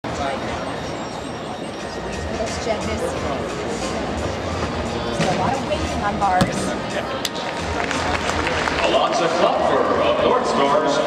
Alonza Klopfer - Vault - 2013 Secret U.S. Classic
Score: 13.450
July 27, 2013 - Sears Centre Arena - Hoffman Estates, Ill.